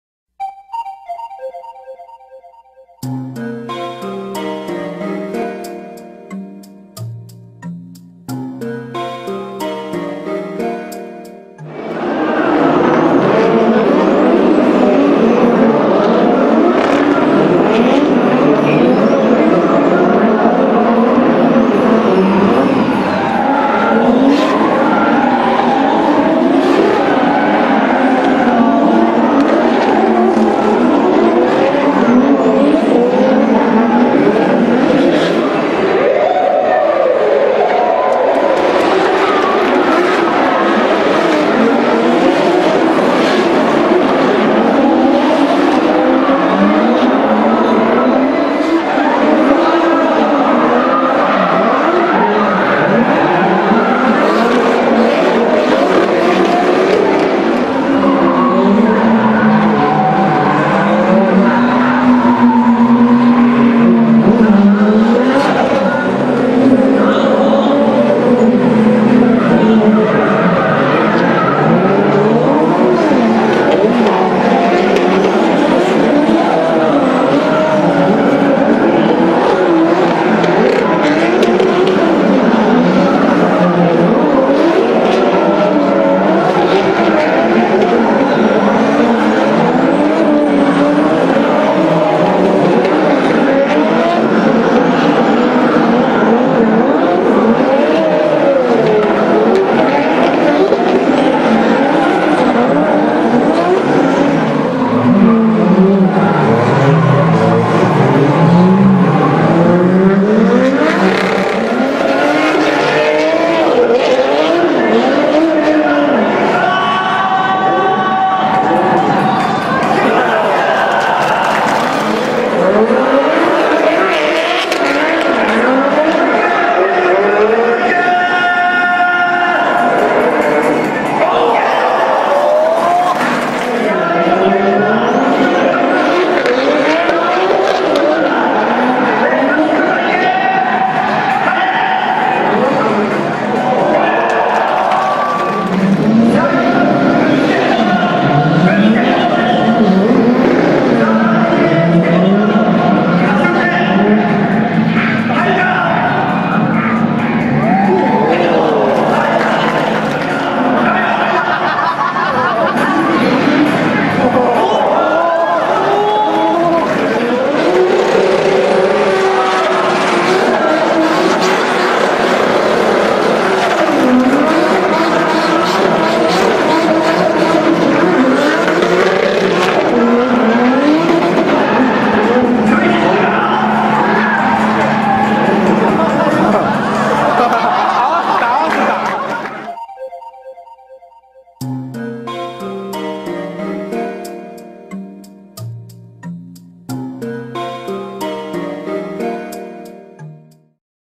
Фигурное вождение от звезд D1 GP.
Дрифт шоу для зрителей перед началом состязаний  на трассе Фуджи Спидвэй в Японии.